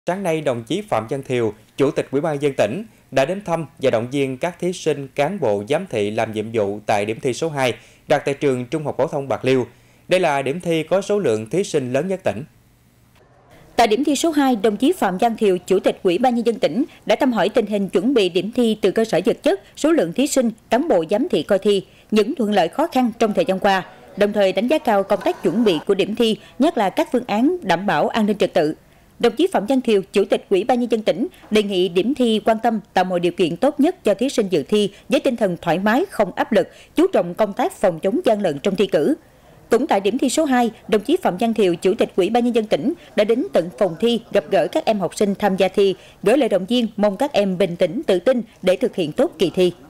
Chủ tịch UBND tỉnh Phạm Văn Thiều thăm điểm thi Tốt nghiệp THPT 2023
Kênh thông tin chính thức của Đài PT-TH Bạc Liêu